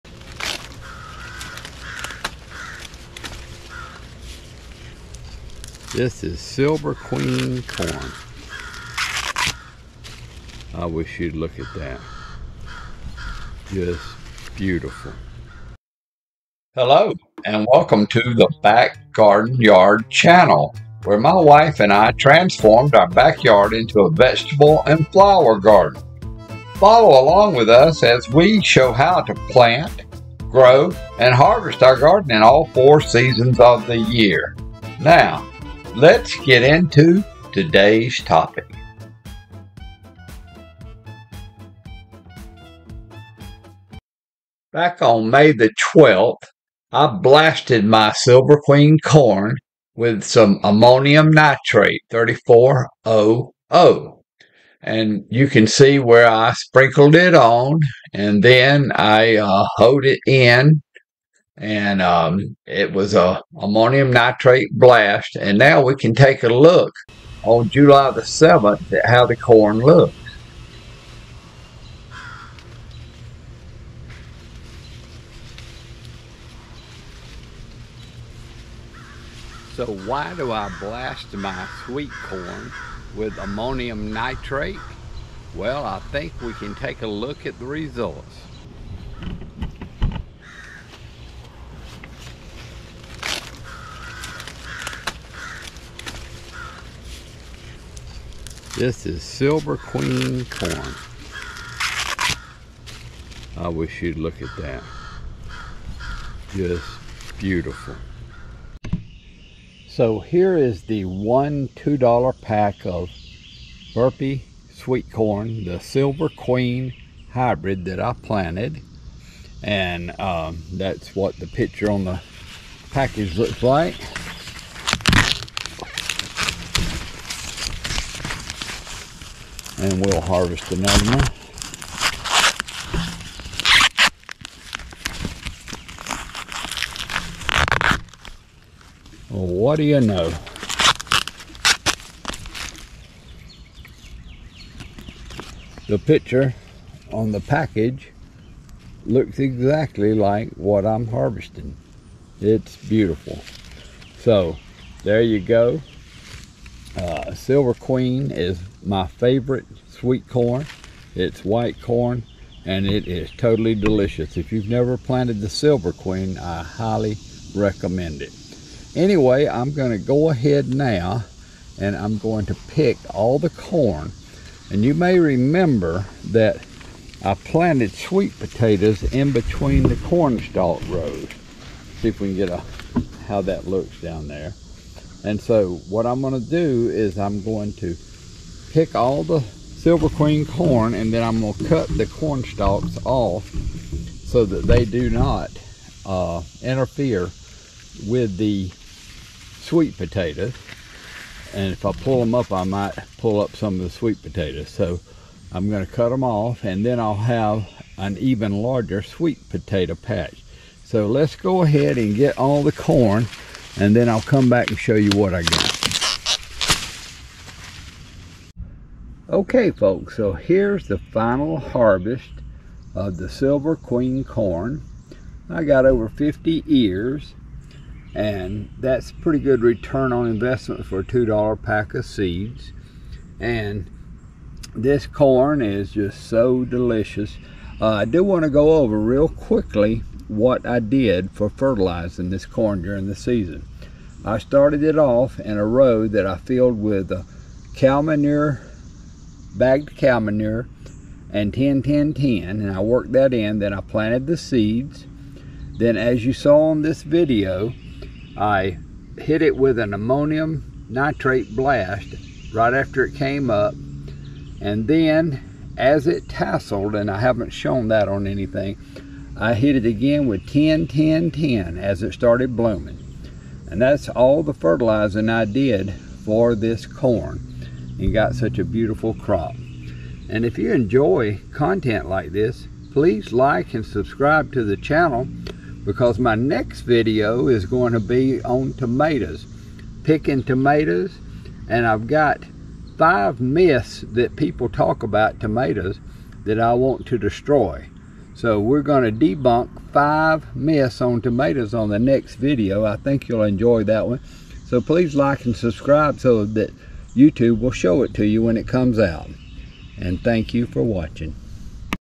I BLASTED MY CORN WITH AMMONIUM NITRATE. HERE'S WHAT HAPPENED!
Ammonium Nitrate is a very misunderstood fertilizer.  I made a video about how to use it in the garden, and here is a
This video shows the final results of using it in the garden on sweet corn.  I also describe the complete use of fertilizers on the corn during the growing season.